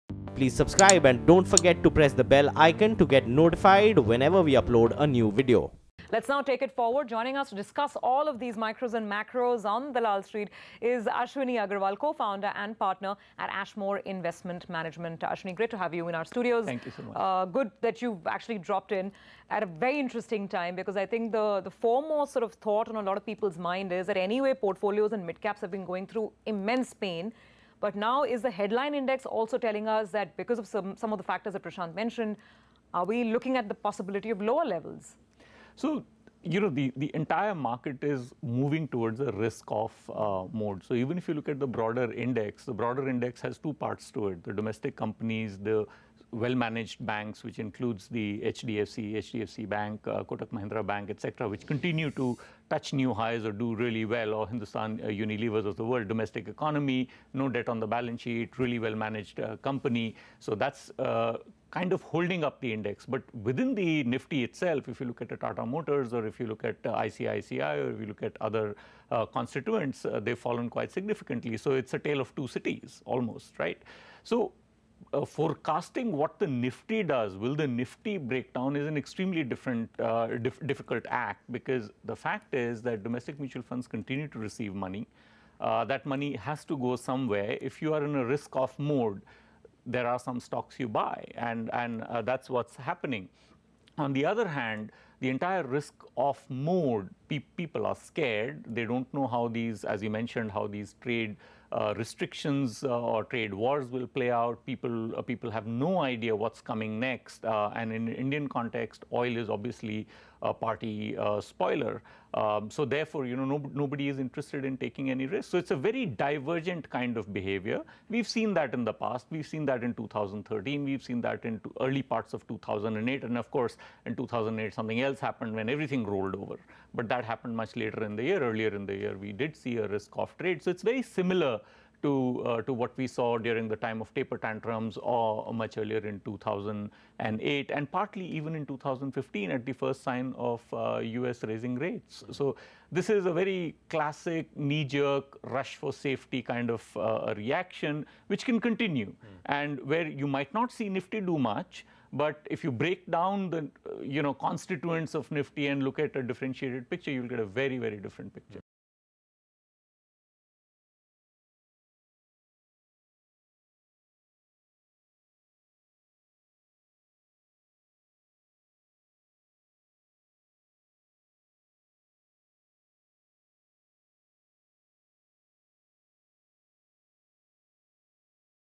Ashwini Agarwal Reviews the Week on Dalal Street | CNBC TV18
Market expert Ashwini Agarwal talks about the current trends and the possible future of the market